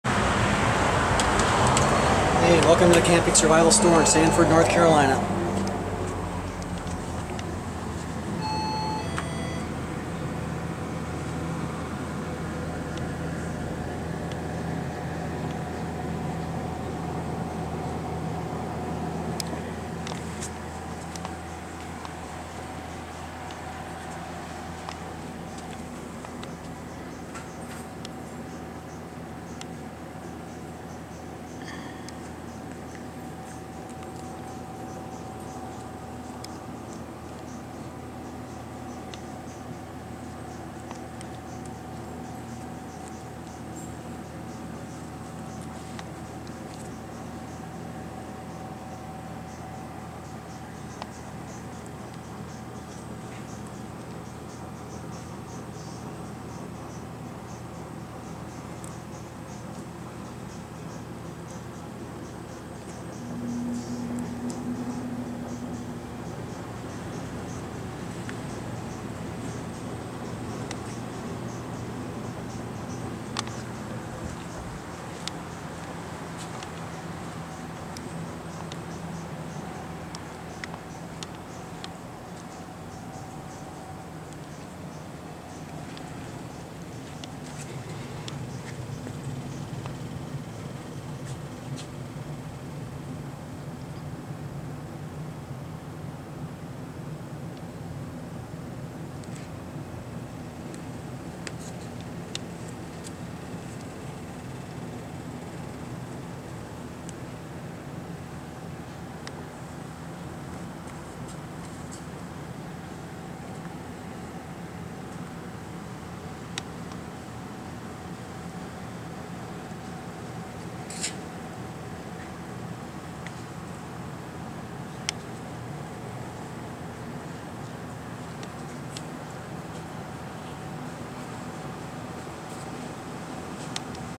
KNIFE PORN ALERT: 2 Minutes in Sanford, North Carolina
All of the knives you see are in our North Carolina store in Sanford, but we have over 2,000 knives our website!